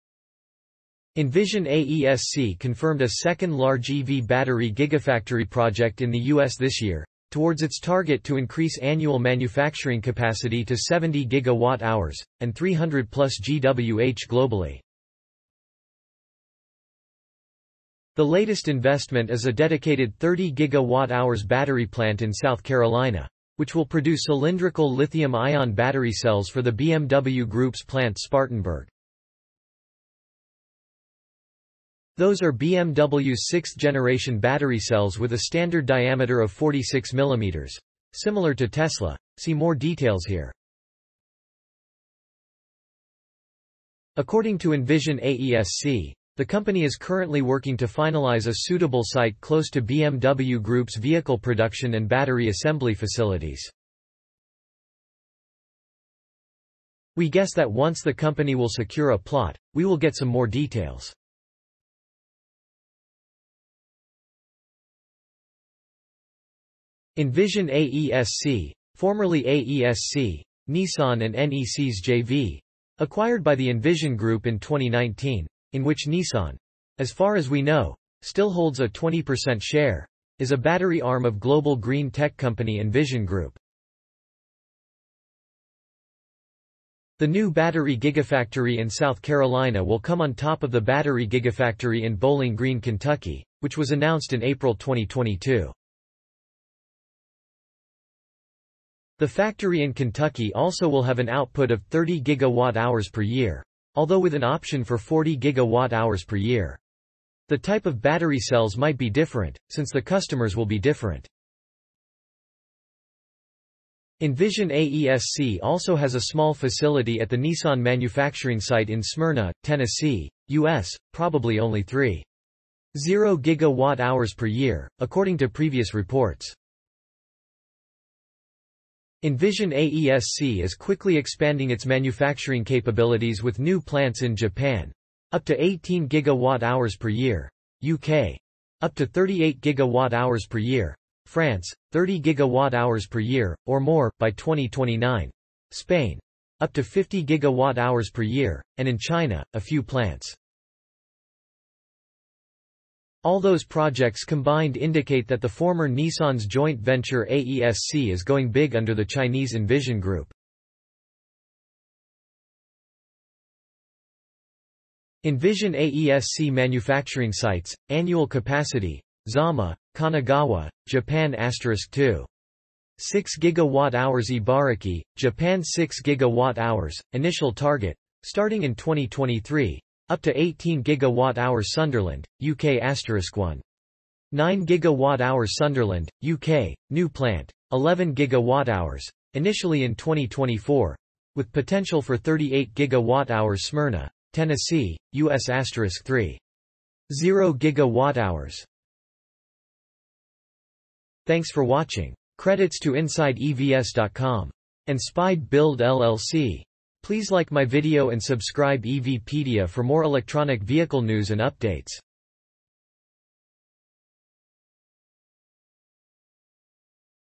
US: Envision AESC Targets 70 GWh/Year Of Battery Manufacturing Capacity
Envision AESC confirmed a second large EV battery gigafactory project in the US this year, towards its target to increase annual manufacturing capacity to 70 GWh (and 300+ GWh globally). The latest investment is a dedicated 30 GWh battery plant in South Carolina, which will produce cylindrical lithium-ion battery cells for the BMW Group's Plant Spartanburg. Those are BMW's sixth-generation battery cells with a standard diameter of 46 mm (similar to Tesla) - see more details here. According to Envision AESC, the company is currently working to finalize a suitable site close to BMW Group’s vehicle production and battery assembly facilities. We guess that once the company will secure a plot, we will get some more details. Envision AESC (formerly AESC - Nissan and NEC's JV - acquired by the Envision Group in 2019, in which Nissan - as far as we know - still holds a 20% share) is a battery arm of global green tech company Envision Group. The new battery gigafactory in South Carolina will come on top of the battery gigafactory in Bowling Green, Kentucky, which was announced in April 2022. The factory in Kentucky also will have an output of 30 GWh per year (although with an option for 40 GWh/year). The type of battery cells might be different, since the customers will be different. Envision AESC also has a small facility at the Nissan manufacturing site in Smyrna, Tennessee, US (probably only 3.0 GWh per year, according to previous reports). Envision AESC is quickly expanding its manufacturing capabilities with new plants in Japan (up to 18 GWh/year), UK (up to 38 GWh/year), France (30 GWh/year "or more" by 2029), Spain (up to 50 GWh/year) and in China (a few plants). All those projects combined indicate that the former Nissan's joint venture AESC is going big under the Chinese Envision Group. Envision AESC manufacturing sites (annual capacity): Zama, Kanagawa, Japan - *2.6 GWh Ibaraki, Japan - 6 GWh (initial target) starting in 2023, up to 18 GWh Sunderland, UK - *1.9 GWh Sunderland, UK (new plant) - 11 GWh (initially in 2024) with potential for 38 GWh Smyrna, Tennessee, US - *3.0 GWh Bowling Green, Warren County, Kentucky, US - 30 GWh starting in 2025 (40 GWh potential) South Carolina, US - 30 GWh (cylindrical battery cells for BMW) Douai, France - 30 GWh "or more" Navalmoral de la Mata, Spain - up to 50 GWh, starting in 2025 (announced in July 2022) Wuxi, China - target 20 GWh Ordos, China - first phase target of **10 GWh Shiyan, China - first phase target of **20 GWh, up to **40 GWh potential Cangzhou, China - 30 GWh starting in 2024 (announced in September 2022) Total: close to 300 GWh/year, assuming all at full capacity * old data (Nissan's era) ** (according to EnergyTrend )